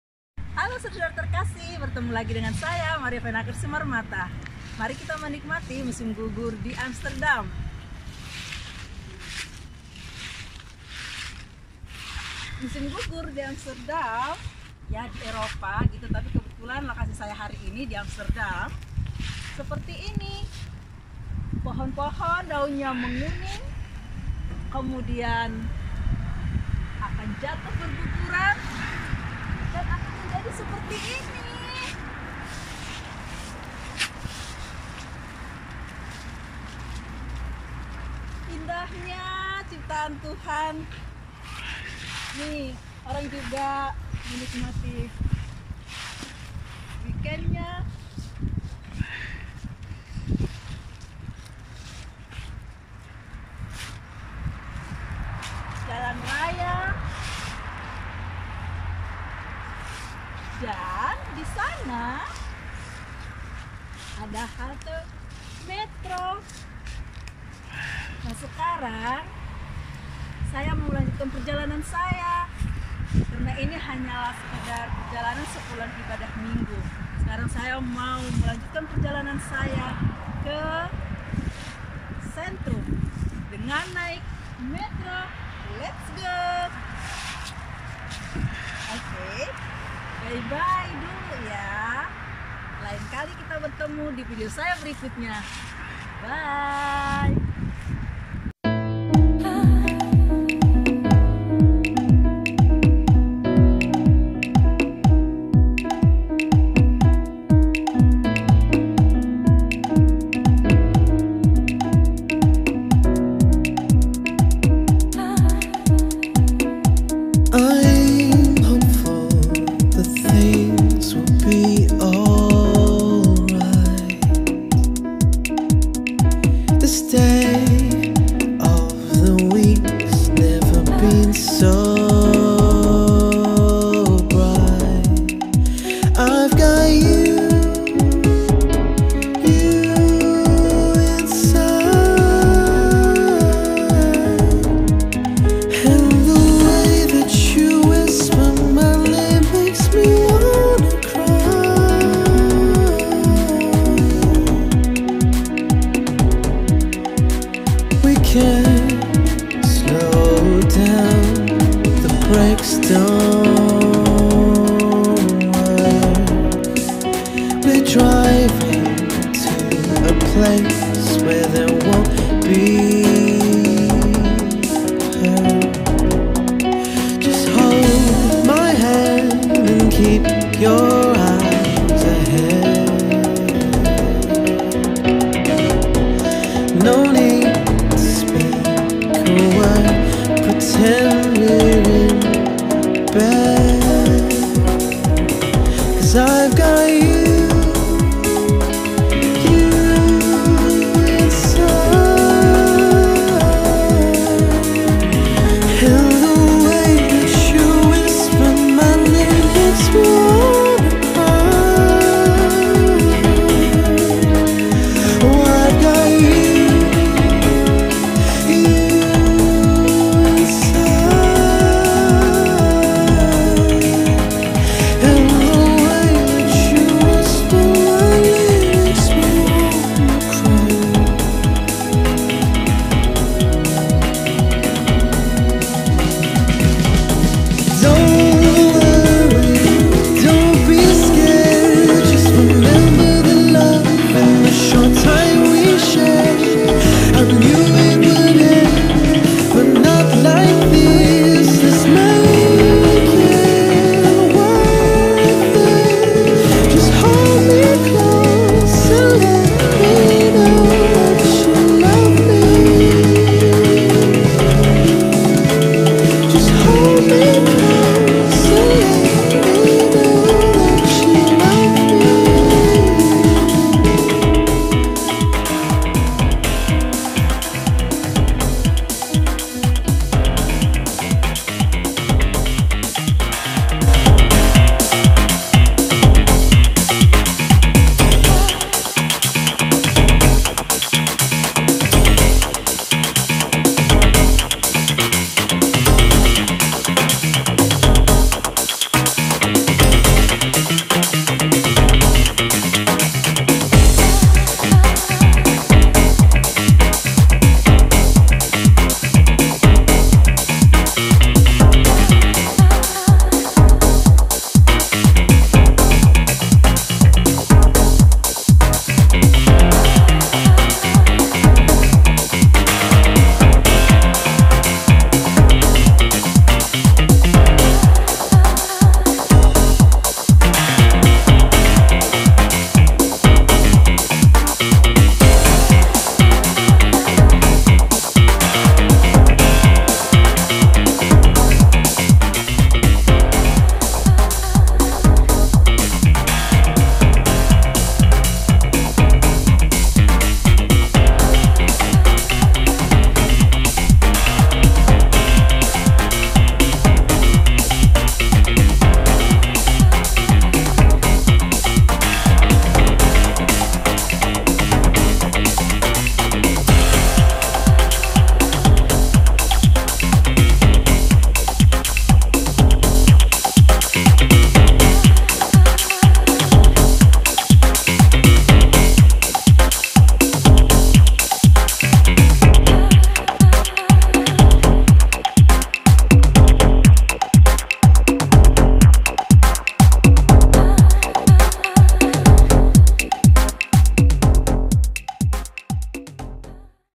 The beauty of Autumn in Holland /Het mooie Herfst in NL /Pesona indahnya musim gugur di Belanda.
I like Autumn so much !
My lovely Autumn memory in Holland with my lovely leople❤️
Mengabadikan kenanganku pada musim gugur di Belanda dengan orang-orang yang ku kasihi dan aku juga mau membagikan keindahananya untuk semua saudara, sahabat dan teman-teman yang terkasih.